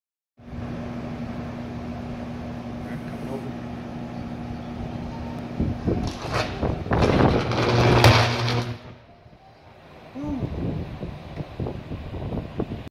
Air break switch opening
115kv switch opening with no load, just line voltage.